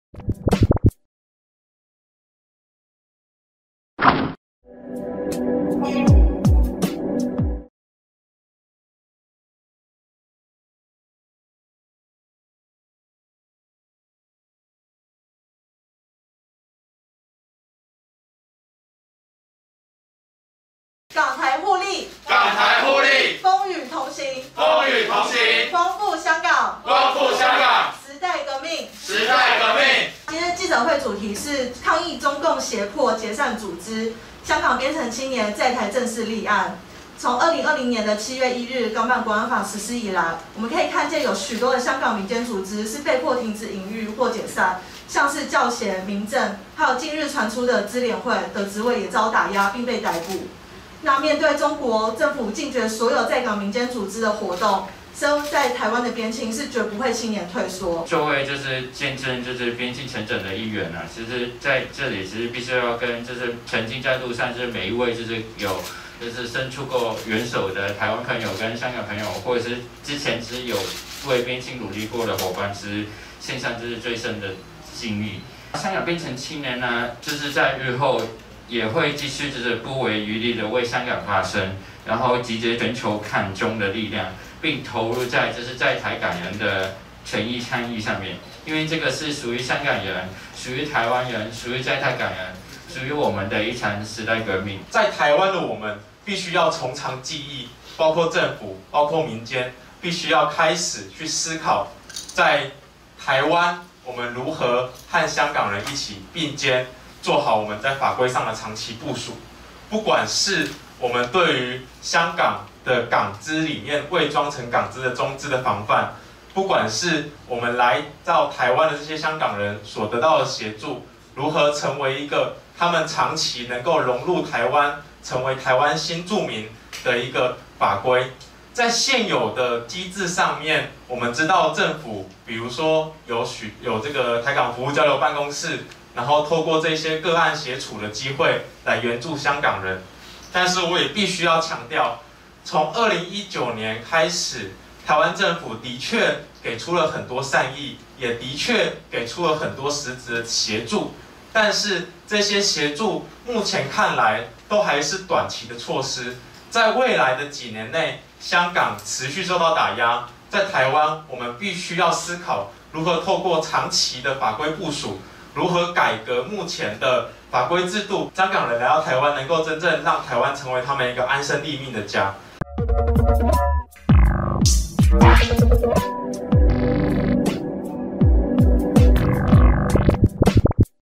2021.09.13香港邊城青年在台正式立案 民團協力撐民主要自由
面對香港民間組織不斷遭受港版《國安法》打壓、被迫解散的情況，香港邊城青年（以下簡稱邊青）宣布在台灣通過內政部登記，正式立案為人民團體。邊青承諾，未來將持續在台推動反中共極權等相關事務。同時，遭受中共極權打壓、被迫來台避難的港人，邊青也會予以幫忙。

了解更多我們關注的議題：
我們經費來自公眾支持，您的相挺，讓我們作更好報導
💖捐贈發票愛心碼：576（我紀錄）